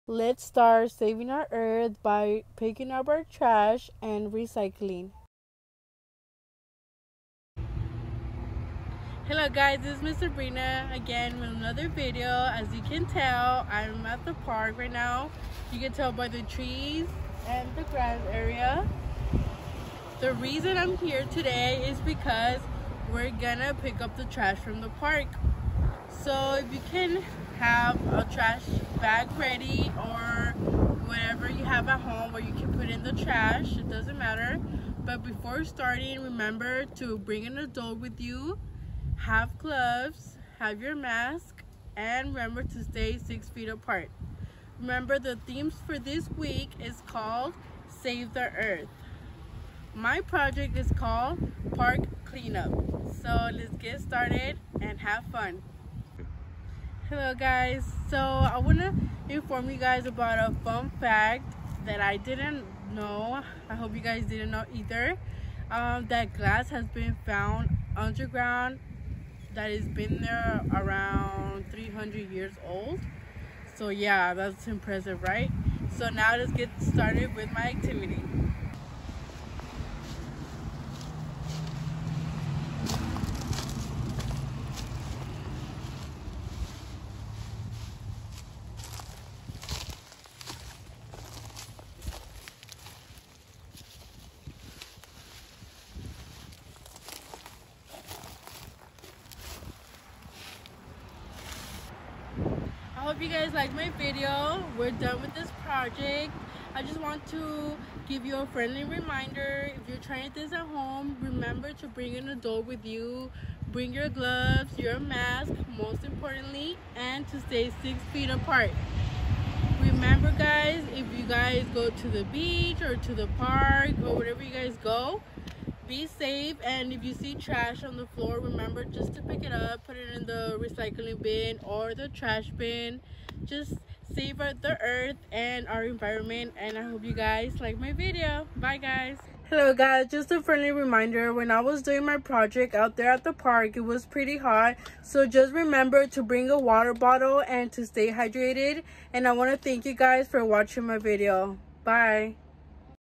Save the Earth: Park Recreational cleanup/Ms.Sabrina
Hello again guys !! This is Ms. Sabrina with another awesome video where I show you another way you can Save the Earth and how we can keep our surroundings clean.Remember if you want to try this home be safe wear a mask , gloves and stay 6ft apart I hope you enjoy my video.

If you have extra time, we would love to hear from you about how we can better our videos and how we can help you more by answering this survey.

Thank you so much, and we appreciate the time you're allotting watching our videos!